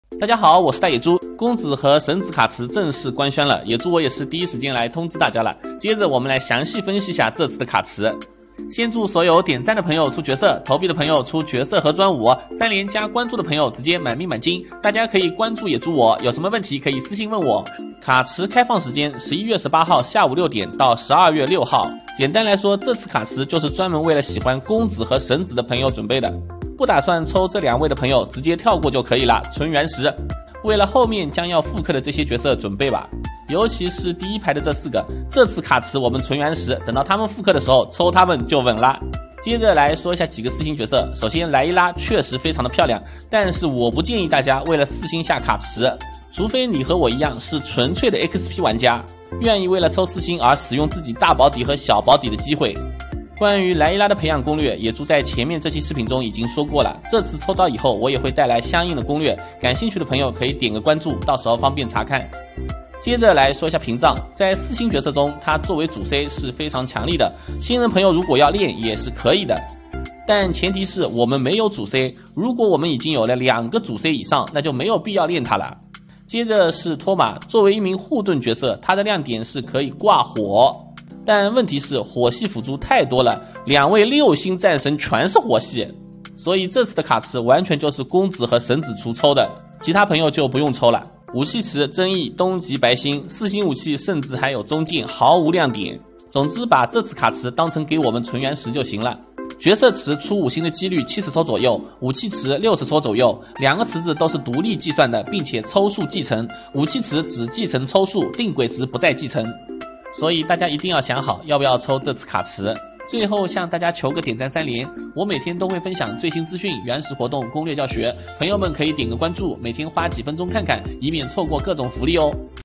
【原神】雷神,胡桃稳了！神子公子卡池官宣！角色详解！抽取建议抽前必看！存原石喽！【自由大野猪】
雷神,胡桃稳了！神子公子卡池官宣！
【原神】雷神,胡桃稳了！神子公子卡池官宣！角色详解！抽取建议抽前必看！存原石喽！【自由大野猪】 请注意，本视频完全符合YouTube社区准则，没有任何违规行为。